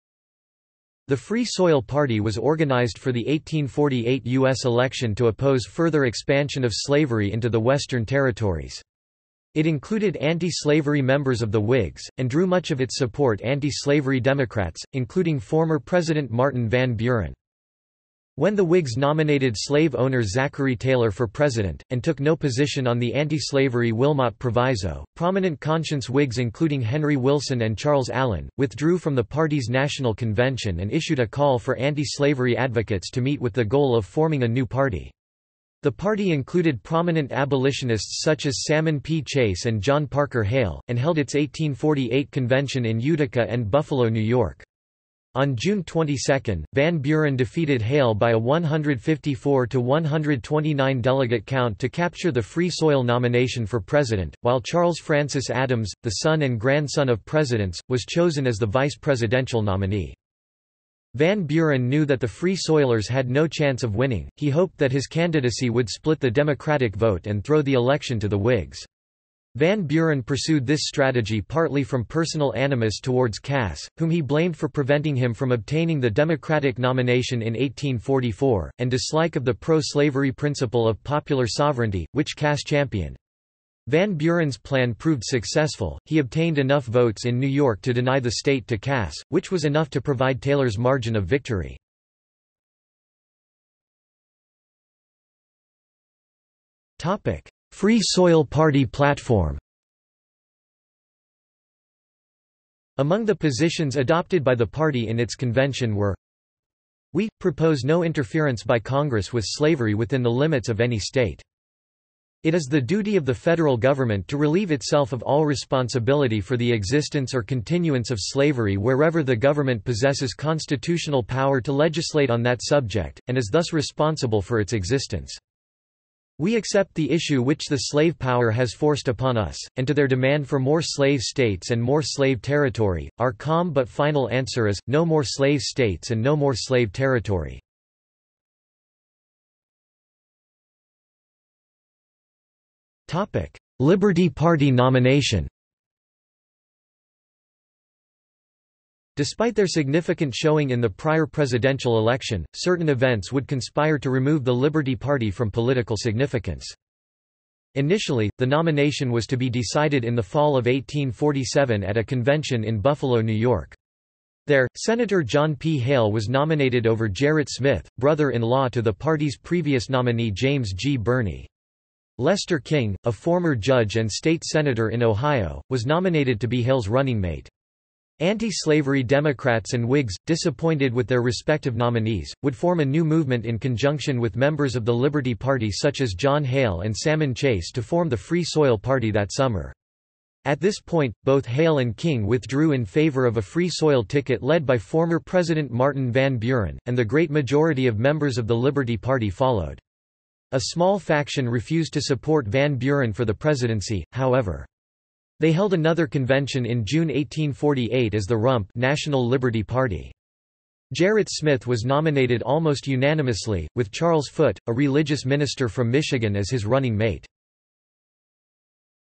1848 Free Soil & Liberty national conventions | Wikipedia audio article
00:01:33 1 Free Soil party platform
00:02:19 2 Liberty Party nomination

- Socrates

SUMMARY
The Free Soil Party was organized for the 1848 US election to oppose further expansion of slavery into the western territories.  It included anti-slavery members of the Whigs, and drew much of its support anti-slavery Democrats, including former President Martin Van Buren.
When the Whigs nominated slave owner Zachary Taylor for president, and took no position on the anti-slavery Wilmot Proviso, prominent Conscience Whigs including Henry Wilson and Charles Allen, withdrew from the party's national convention and issued a call for anti-slavery advocates to meet with the goal of forming a new party.  The party included prominent abolitionists such as Salmon P. Chase and John Parker Hale, and held its 1848 convention in Utica and Buffalo, New York.  On June 22, Van Buren defeated Hale by a 154-129 delegate count to capture the Free Soil nomination for president, while Charles Francis Adams, the son and grandson of presidents, was chosen as the vice-presidential nominee.
Van Buren knew that the Free Soilers had no chance of winning; he hoped that his candidacy would split the Democratic vote and throw the election to the Whigs.  Van Buren pursued this strategy partly from personal animus towards Cass, whom he blamed for preventing him from obtaining the Democratic nomination in 1844, and dislike of the pro-slavery principle of popular sovereignty, which Cass championed.  Van Buren's plan proved successful; he obtained enough votes in New York to deny the state to Cass, which was enough to provide Taylor's margin of victory.